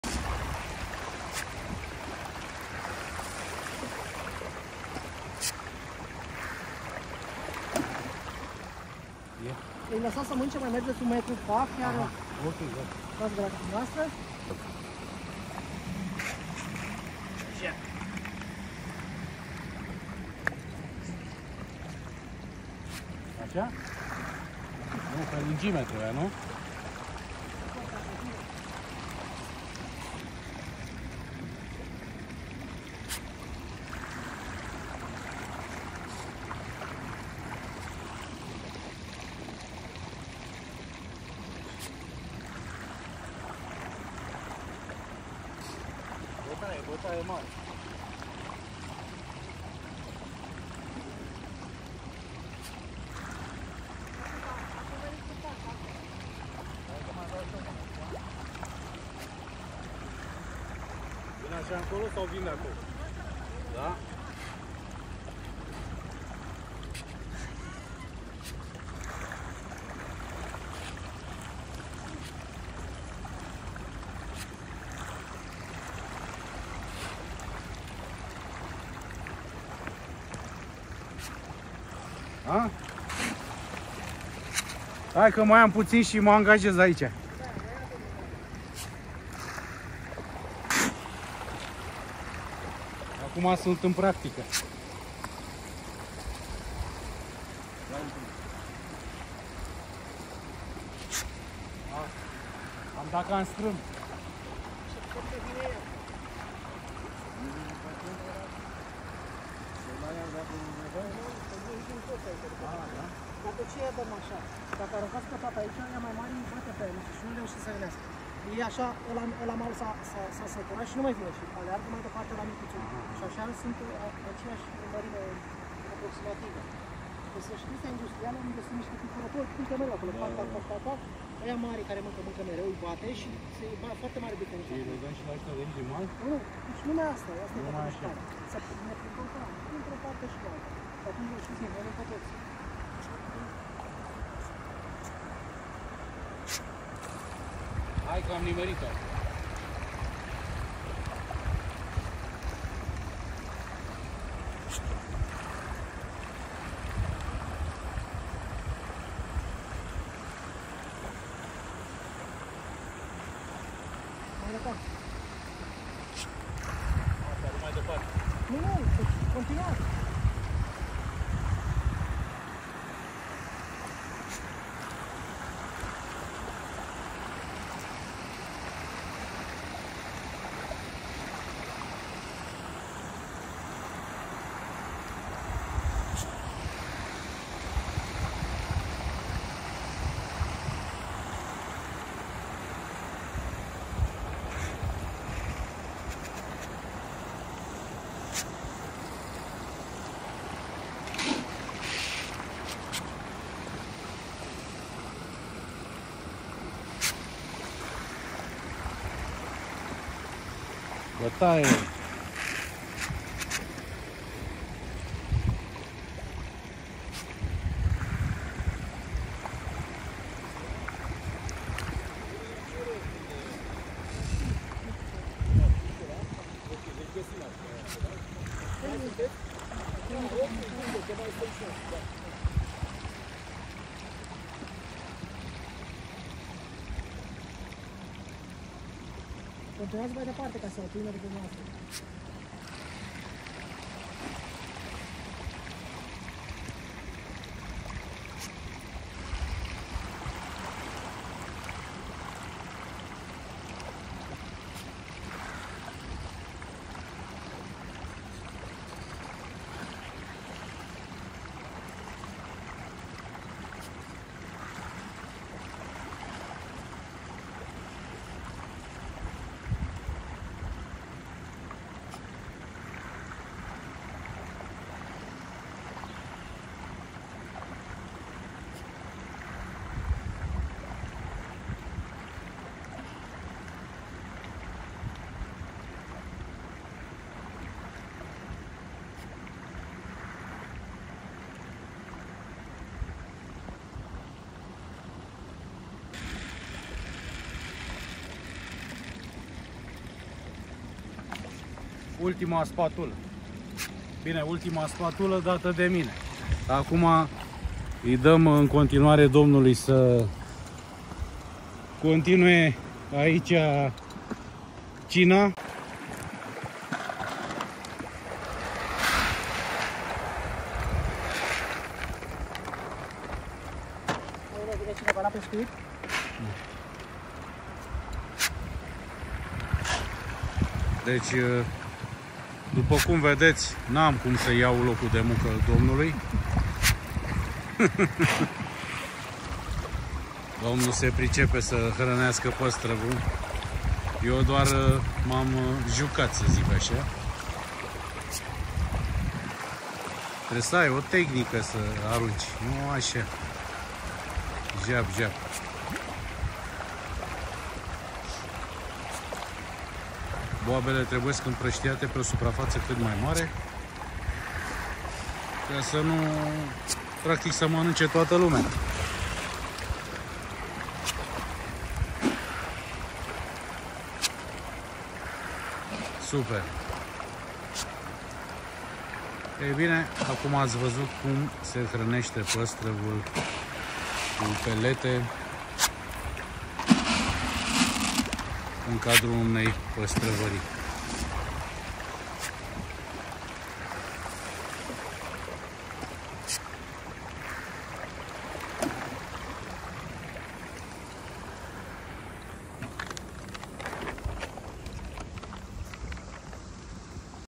Hrănesc 10000 de PĂSTRĂVI
Cum se hraneste PASTRAVUL?
Am asistat la masa de seara a pastravului in cadrul Pastravariei Zavoi. Am zis ca este nteresant sa vedeti si voi cum se hraneste pastravul intr-o pastravarie, asa ca am surprins cateva imagini si va invit sa vedeti!